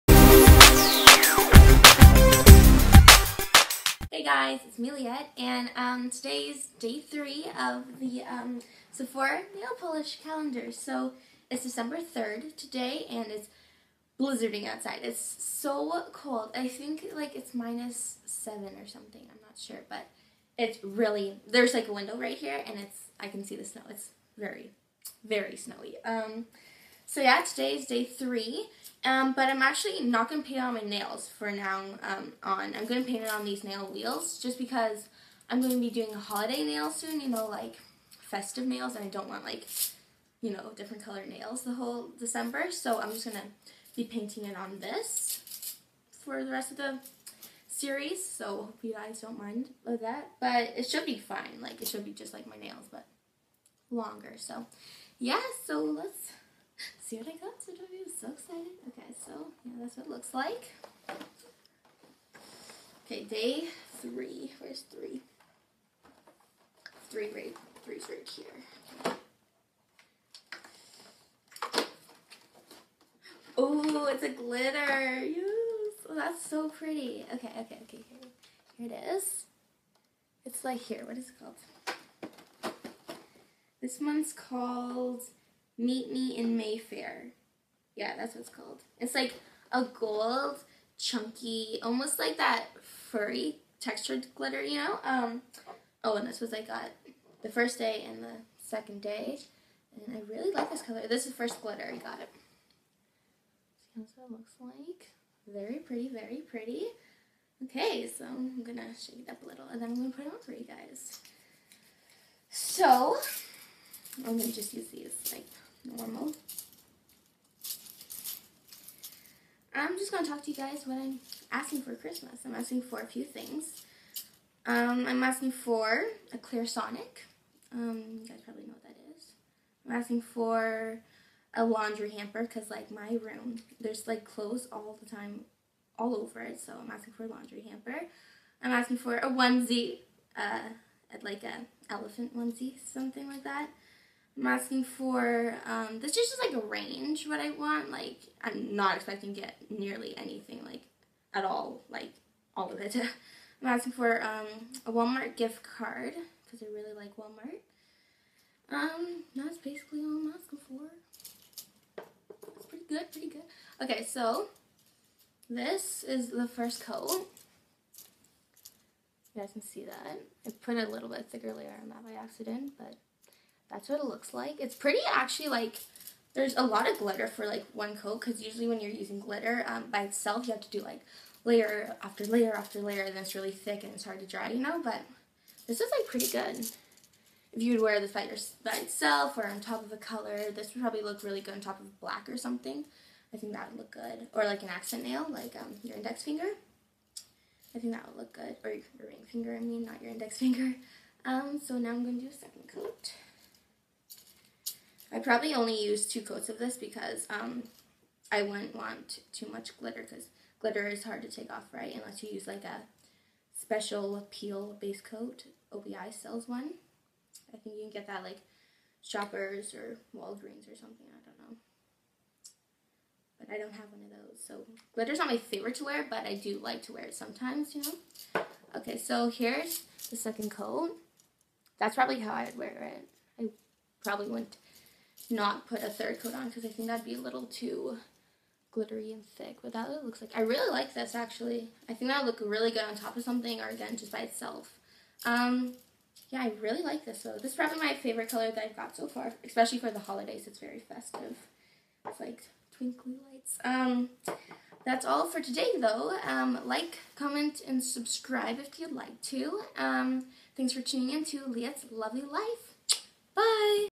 24 DAYS OF SEPHORA NAIL POLISH (DAY 3) + WHAT I WANT FOR CHRISTMAS!
Hey guys thanks for stopping in to watch the third video in my Sephora nail polish series!
Here are the links to the first two videos in the series, enjoy!